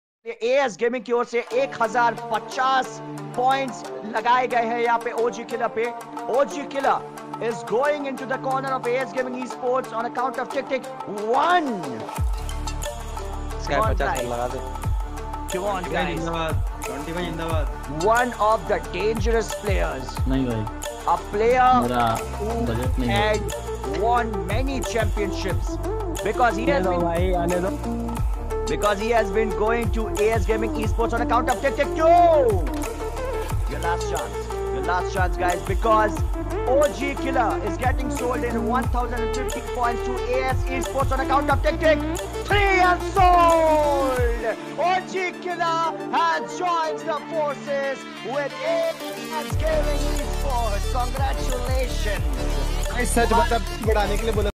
OG KILLER ON AUCTION😎😳 //HELLRAISER INVITATIONAL AUCTION
CREDIT - critical X

HELLO GUYS THIS AUCTION IS ORGANISED BY LOCO TV IN THIS AUCTION THEIR ARE 18 TEAMS OF ALL BIG YOUTUBERS EVERY YOUTUBER IS GOT 2000 COIN TO BUY PLAYERS.

 THIS IS THE LOGIC OF THIS AUCTION!